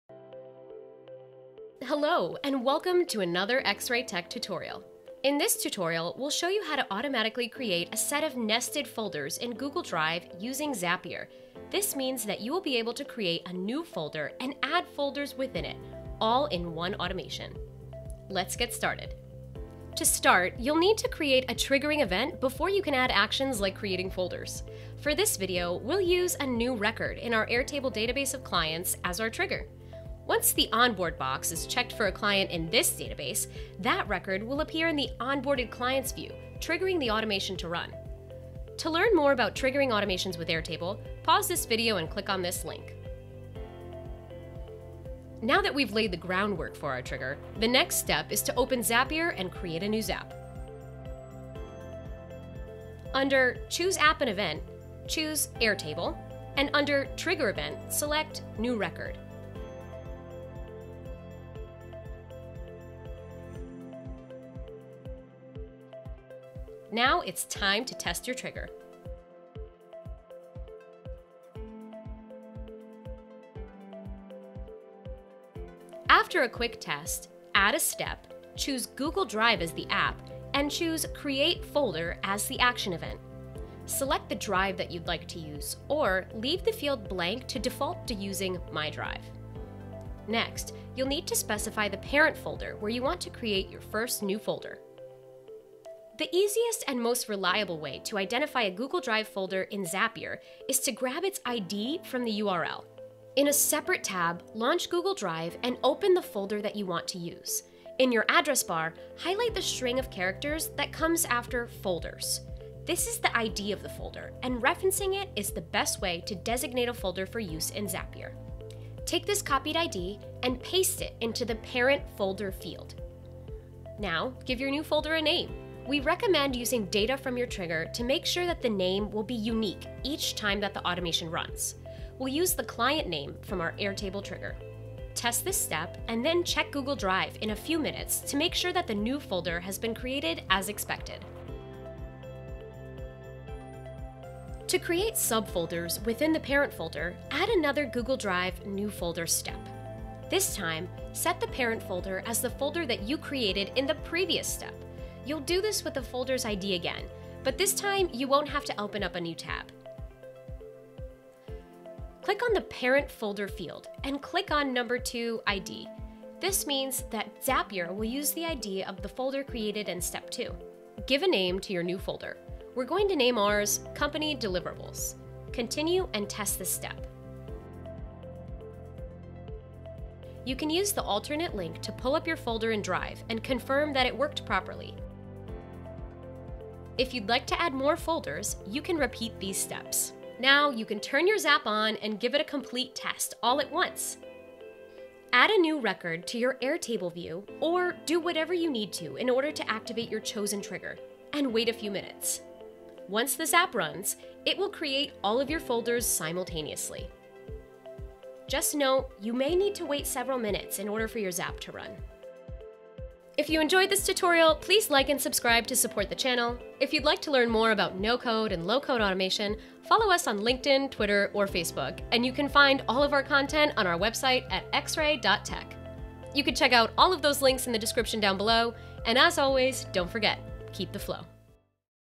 How to Make Nested Folders Automatically in Google Drive with Zapier
With this simple no-code automation, you can instantly make any Google Drive folder structure you want to include in your automated workflows.

Read our blog posts for tutorials and deep dives into automation

Check out our Zapier Experts page for reviews and more information about XRay